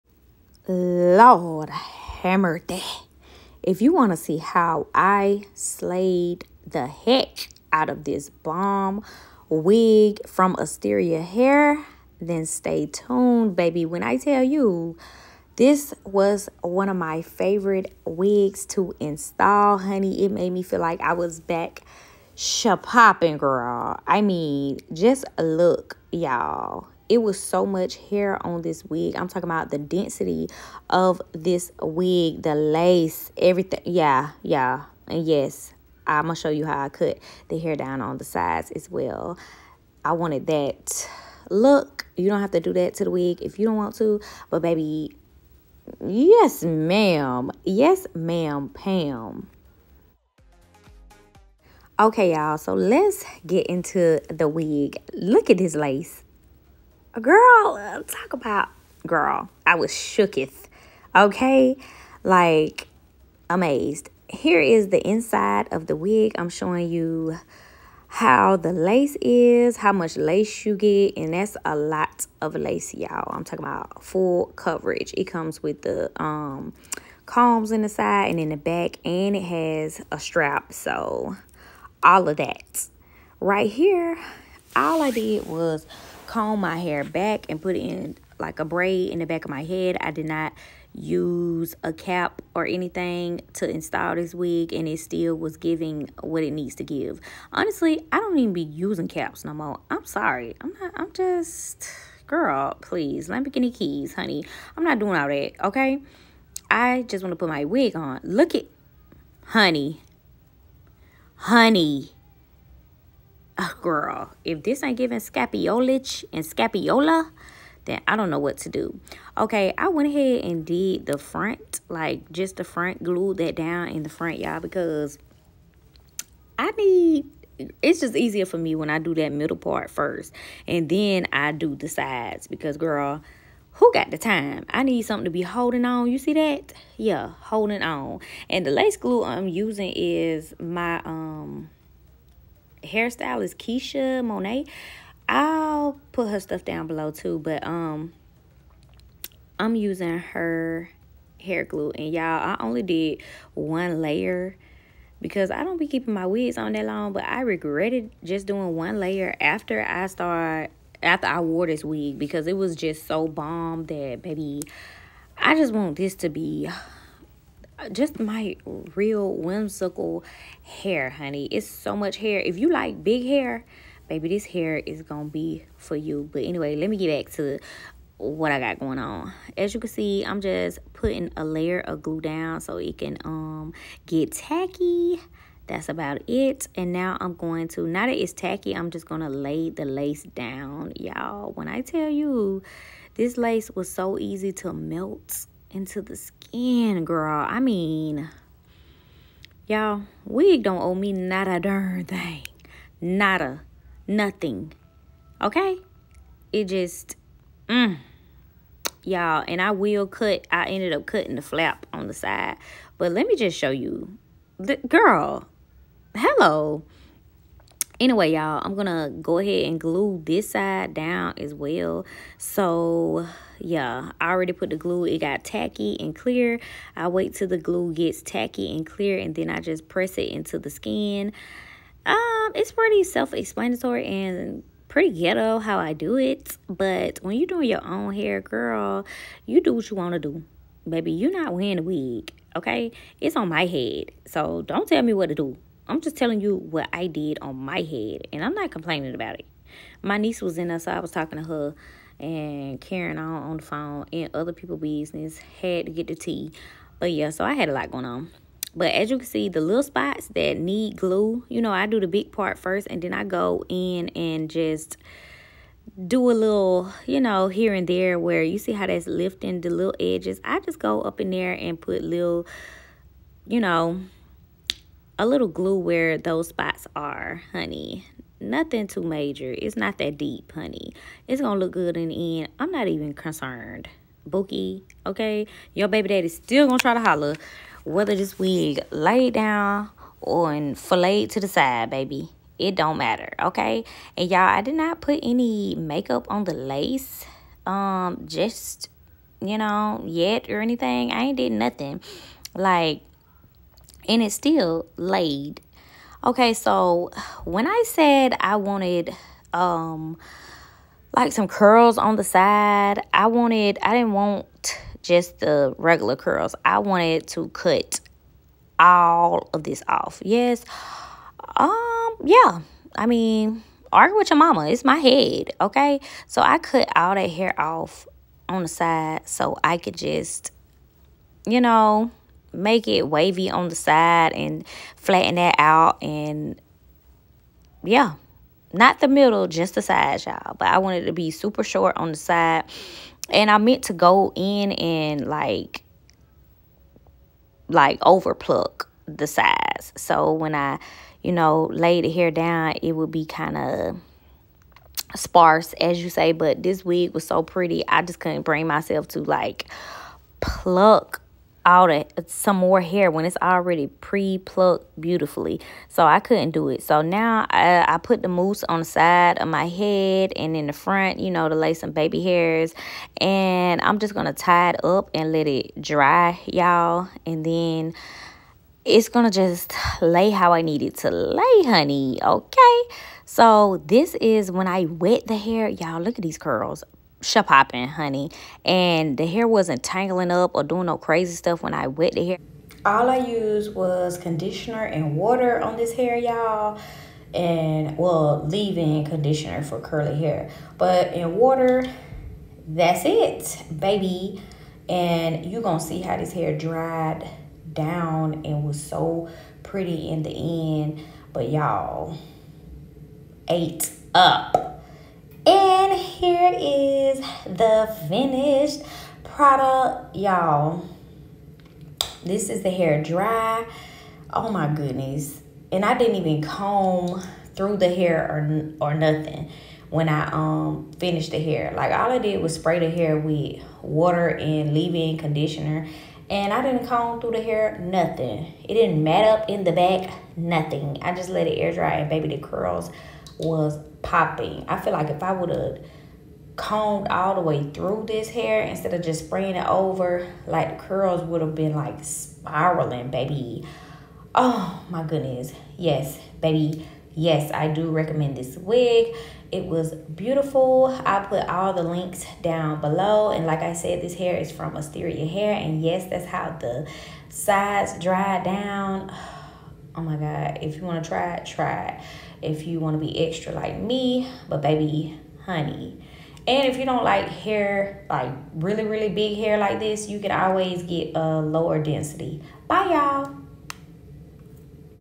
Curls Popping! My Favorite Curly Wig Install With Fluffy Baby Hair (Talk Through) Ft. Asteria Hair
Hair Info: 26 inch curly 13x4 HD lace front wig 250% density

▬▬▬ Asteria Hair 7th Anniversary Celebration ▬▬▬
UP TO $85 OFF, CODE: 7year